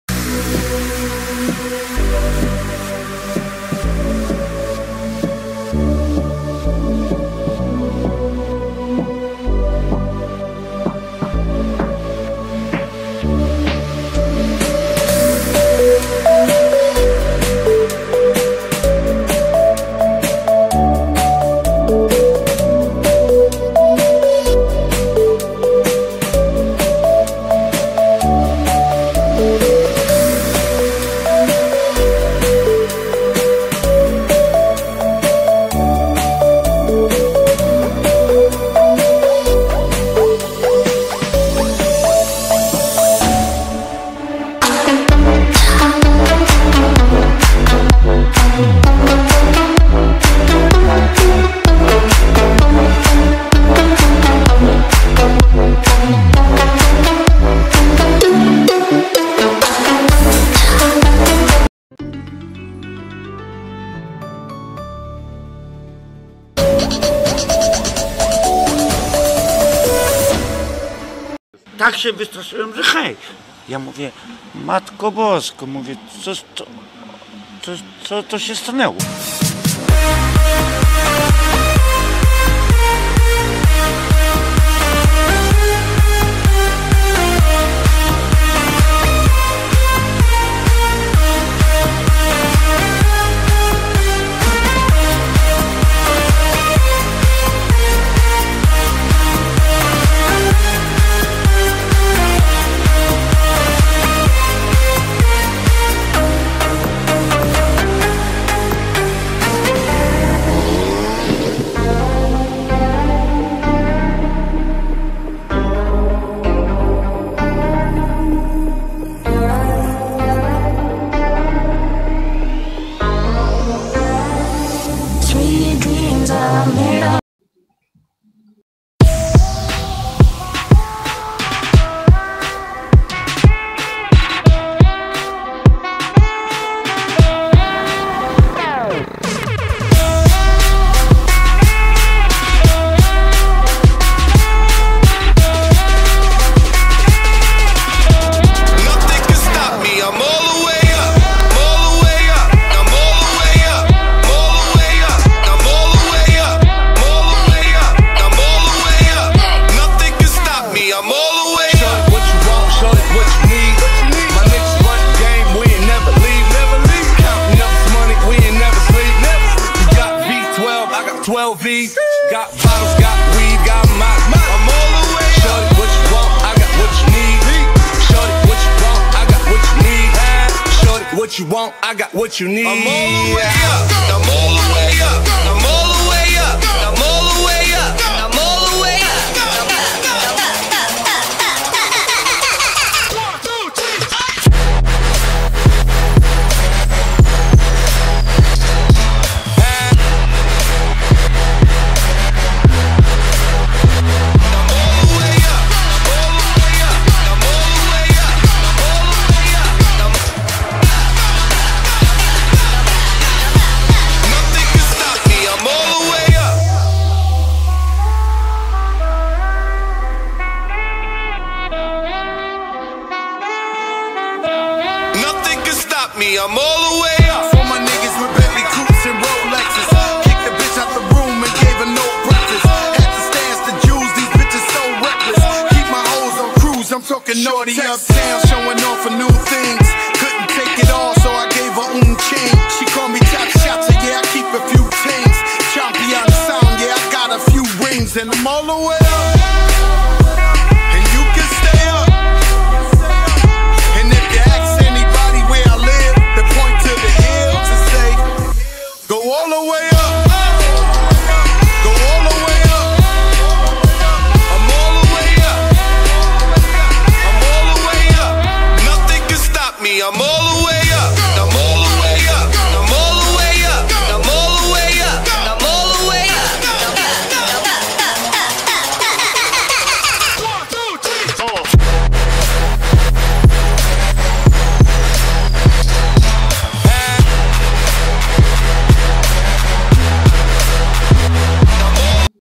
Podsumowanie sezonu 2019 *HARD ENDURO R.I.P*
Podsumowanie sezonu 2019! Jeśli film się podoba zapraszam do zostawienia łapki w górę oraz subskrypcji!
Przepraszam za jakość ale filmiki były przesyłane kilkukrotnie co spowodowało popsucie jakości.